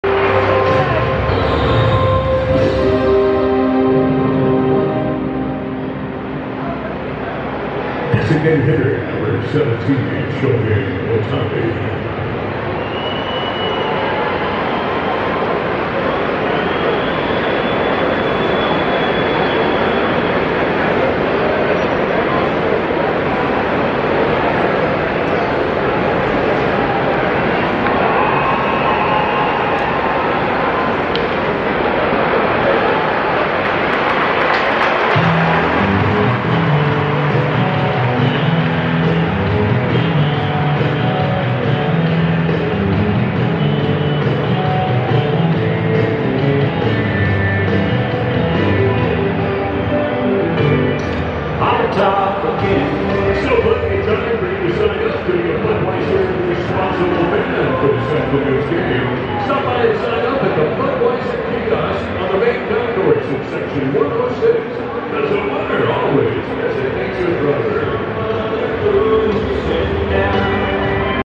速報！元女房に嫌気をさす大谷翔平 第2打席【7.2現地映像】
エンゼルス1-5アストロズ
3番DH大谷翔平　3回表2アウトランナーなし場面